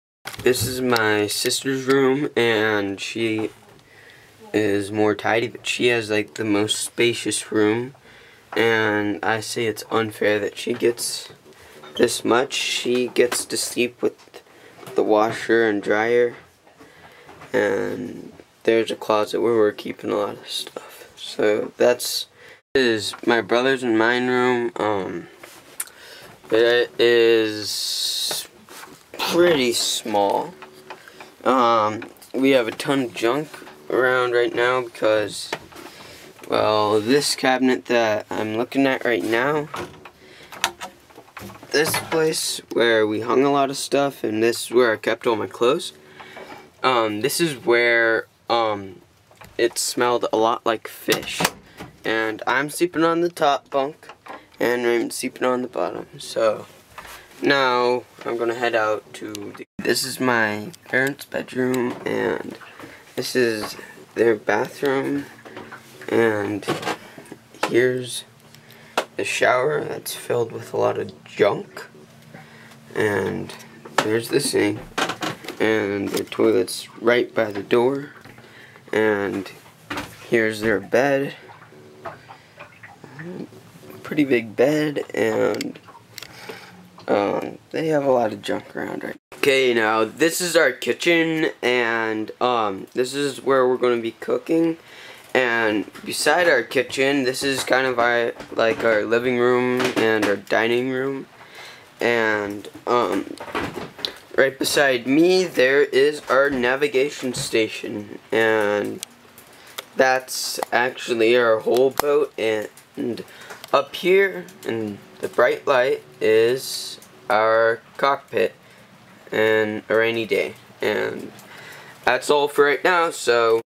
New Project
Our sail boat that will be our home for our around the world trip.